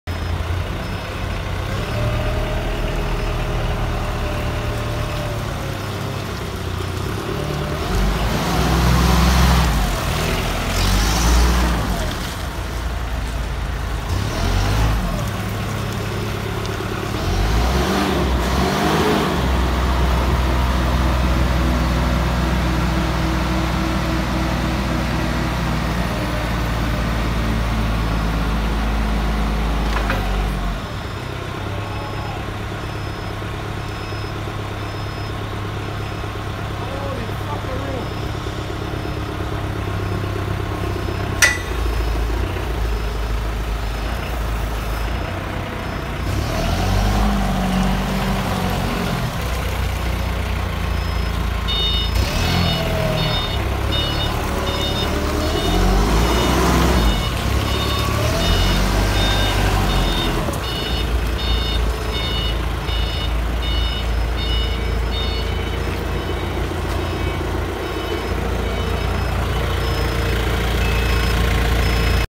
Value diesel forklift with dually
This is a 7000 lb diesel Value forklift with dually front tires on it. This has solid pneumatic tires, LED lights, etc.